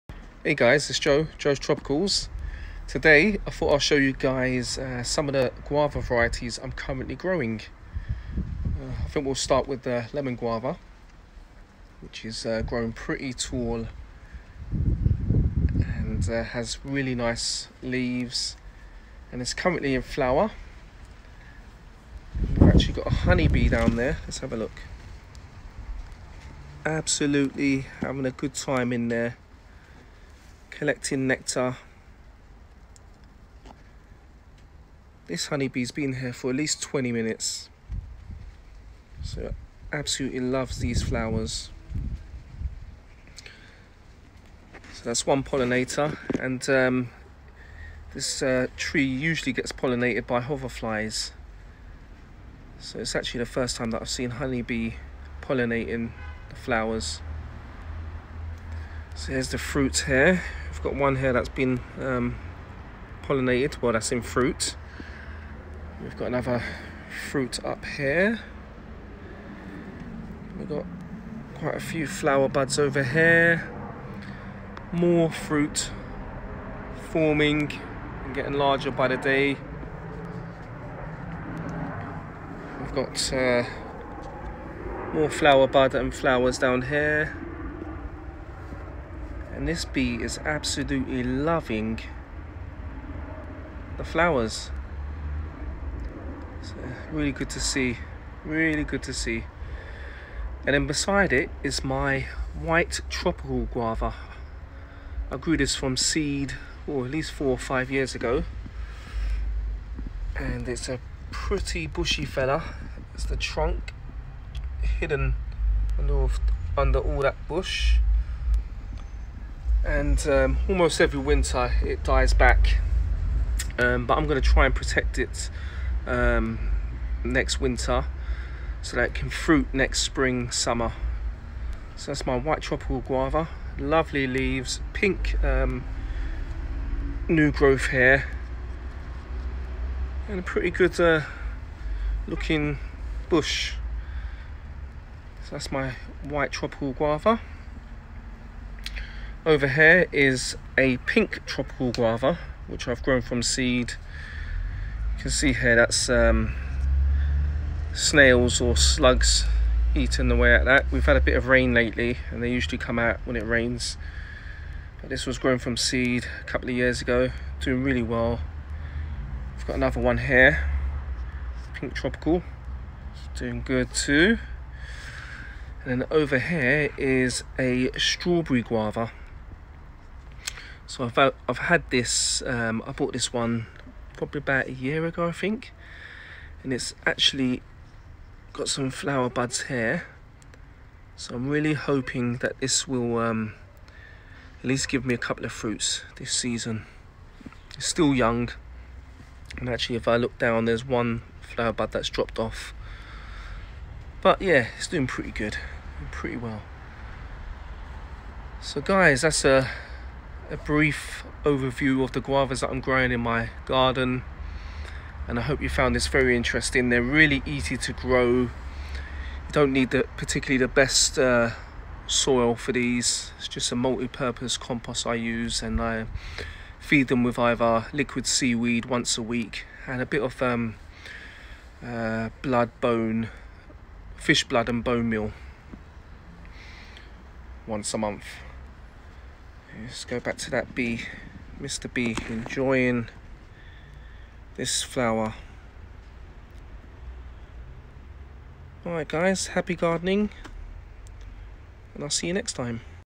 My Guava varieties 9th July 2020
I am currently growing 4 varieties of Guava. Some are more cold tolerant than others.